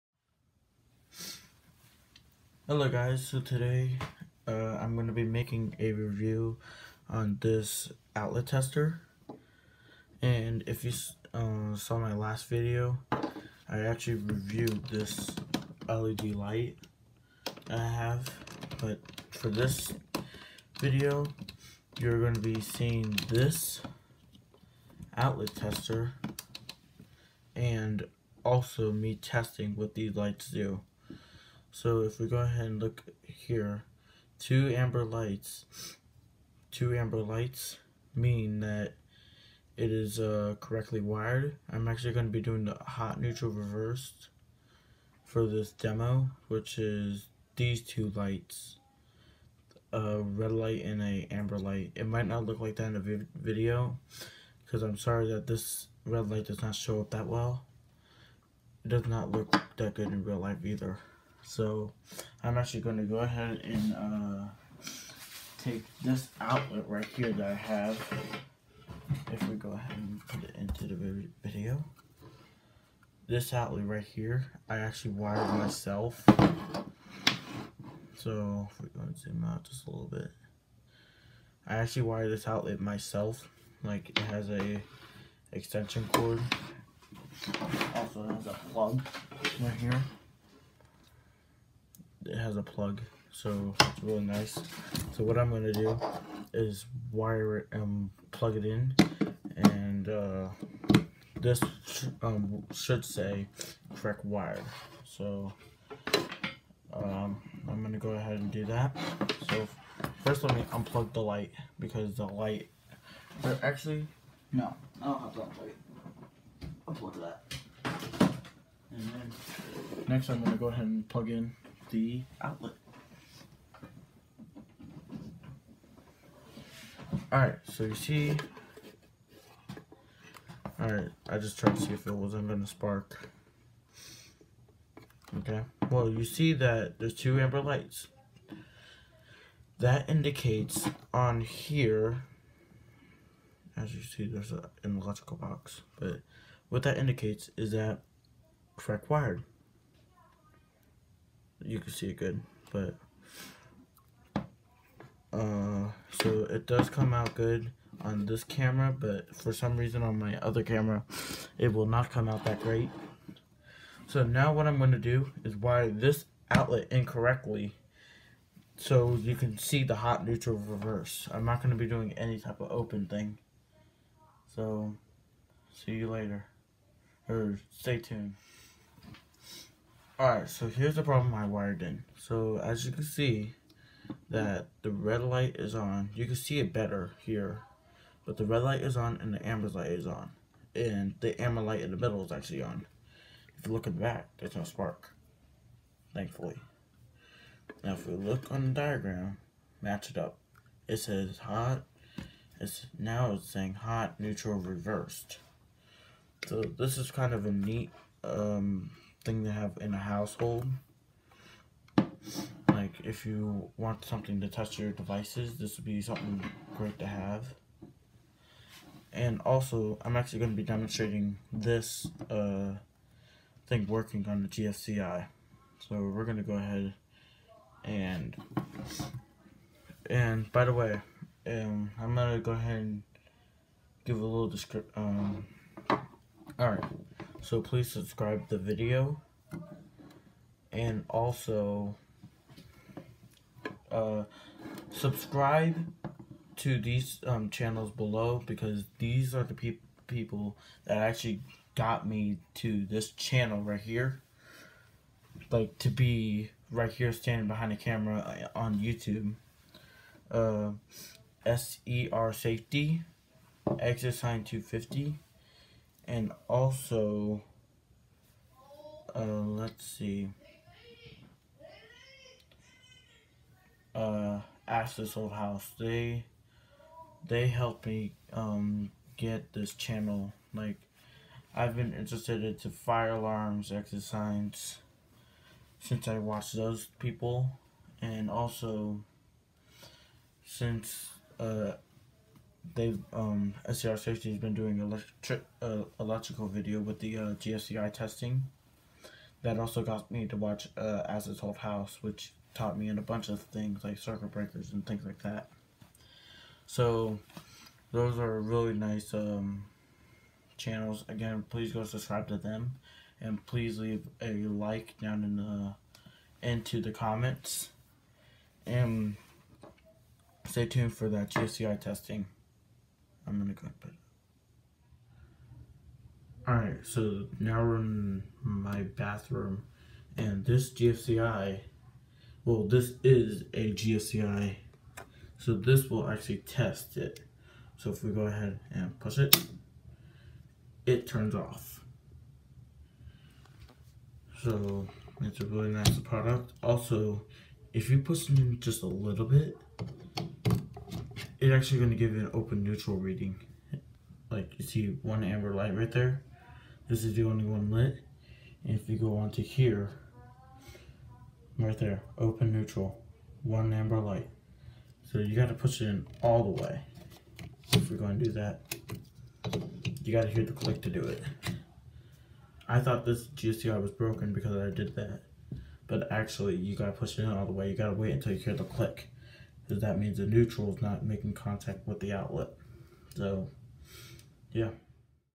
Review on a Gardner bender gfci outlet testers
In this video I will be showing and overviewing a outlet tester and some of the troubles of miss wiring